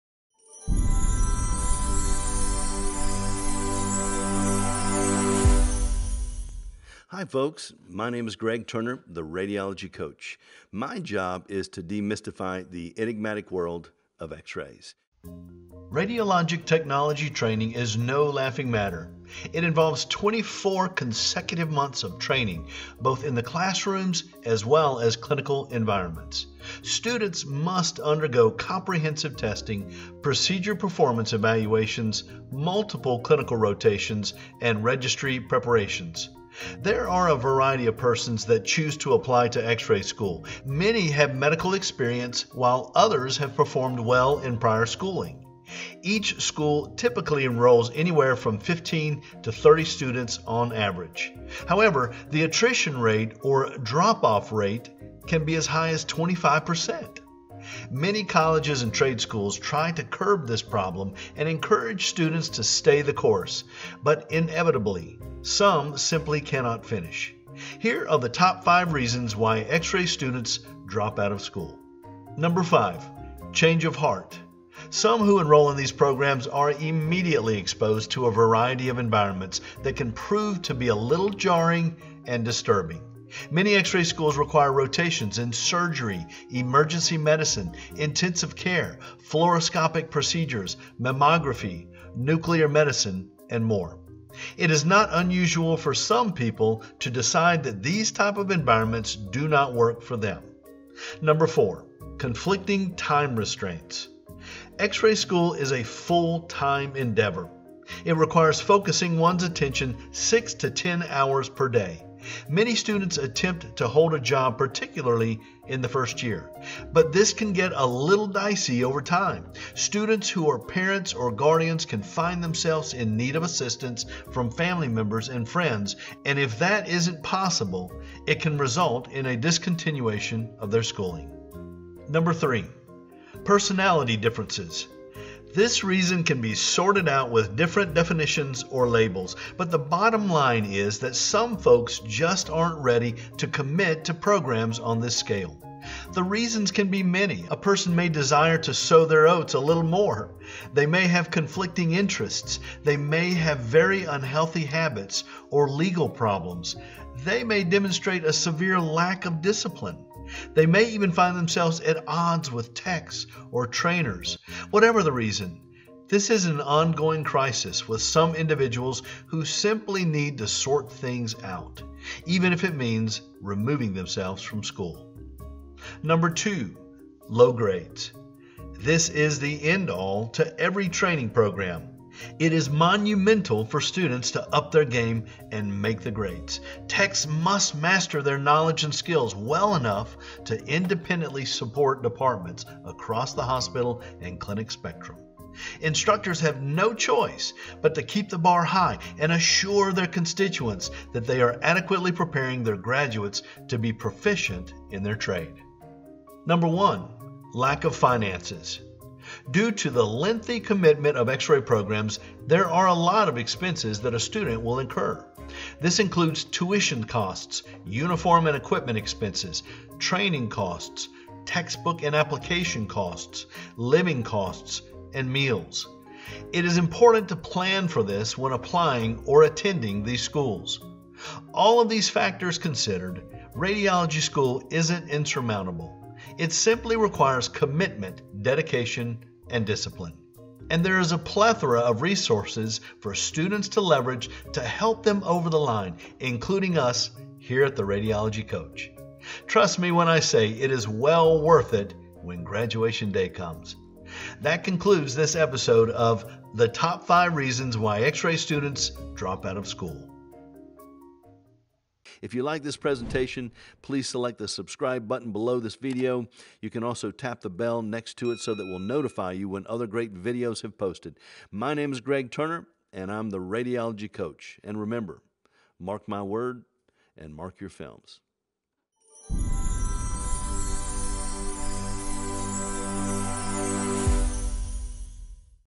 The Top 5 Reasons Why X-RAY Students DROP OUT of School!!
Radiologic Technology school doesn't come easy. It has its pitfalls and challenges. Here are a few reasons why some students fail to complete their studies.